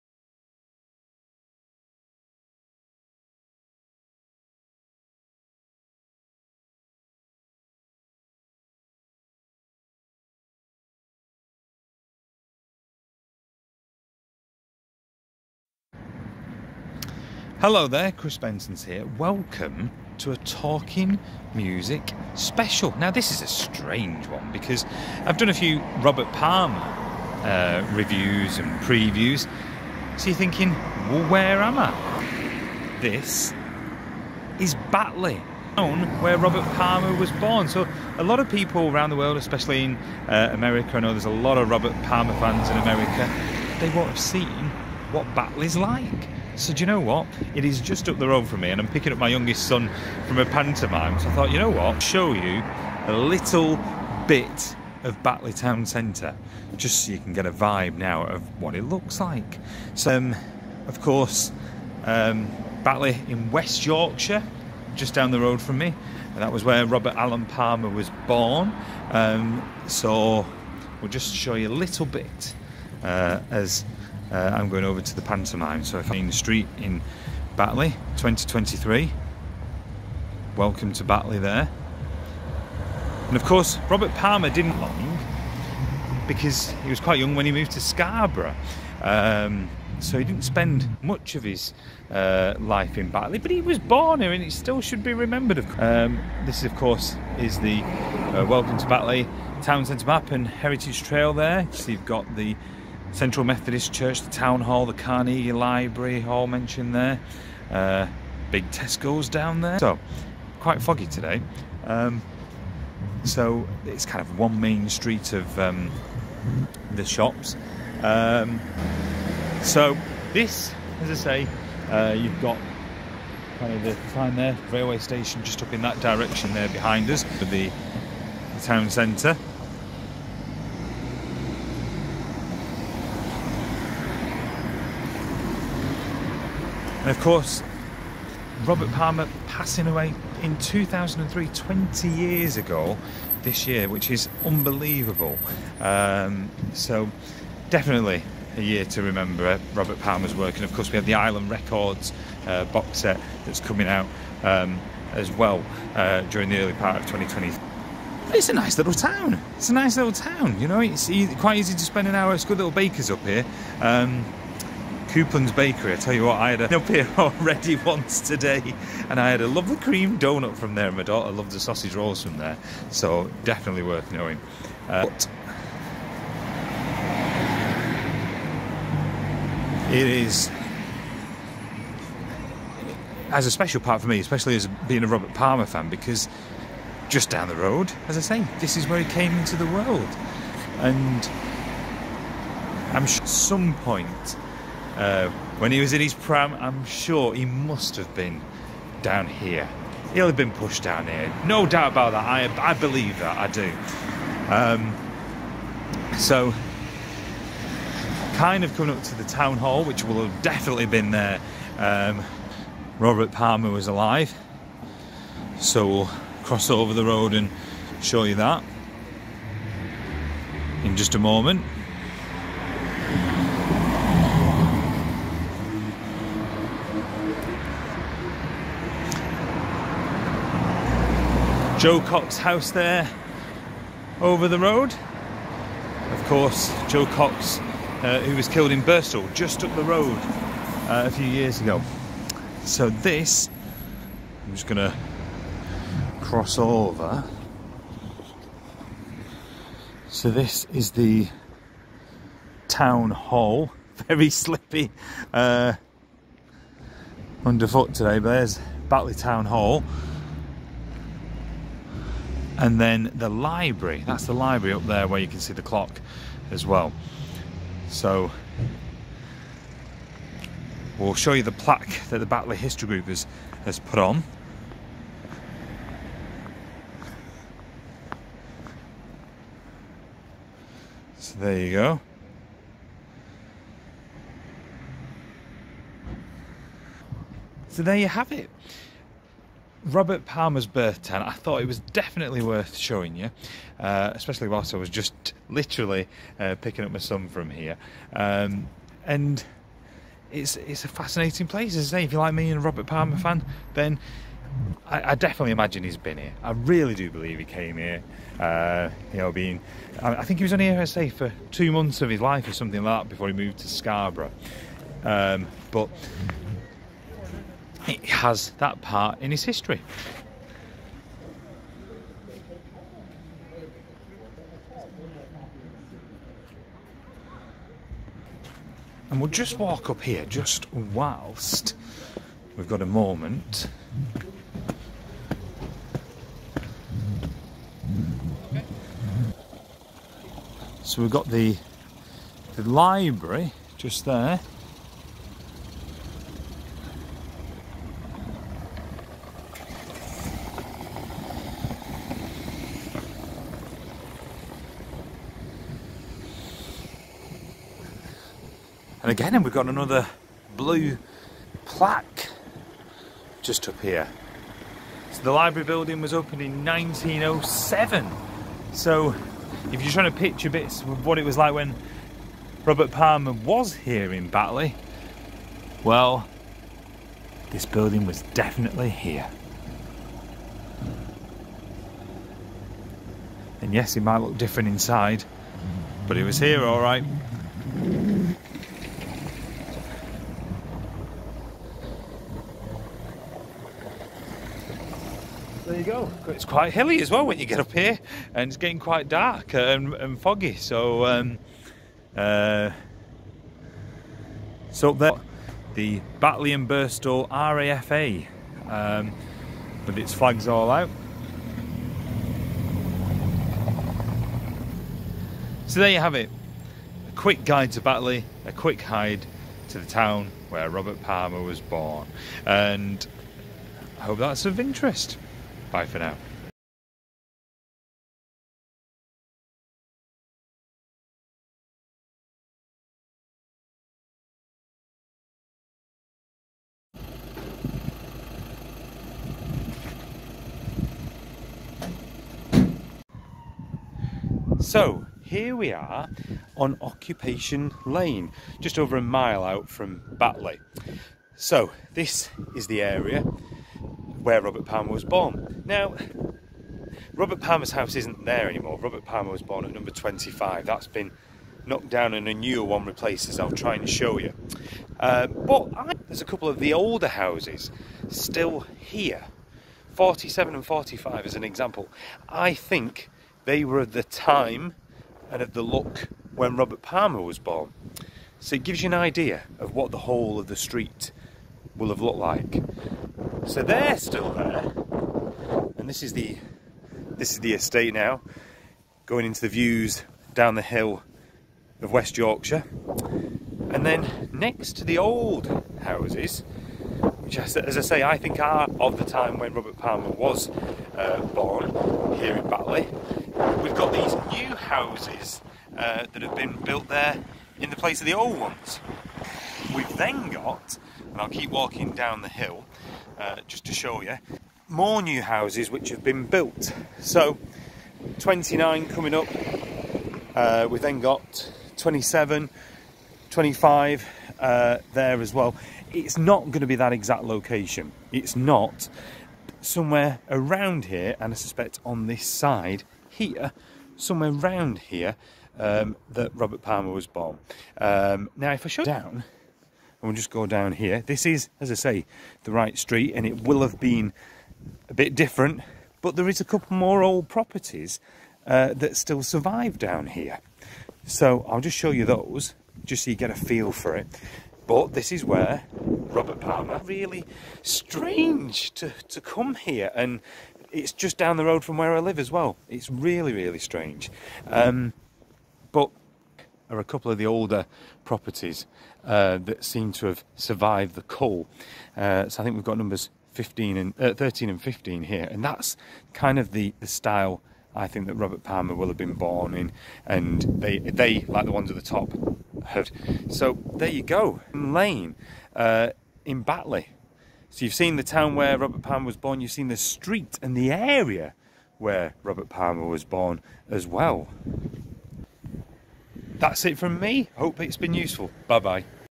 Talking Music - Robert Palmer's Birthplace - Visiting Batley, West Yorkshire
Chris Benson visits Batley Town Centre and Occupation Lane in Batley, West Yorkshire where Robert Palmer was born in 1949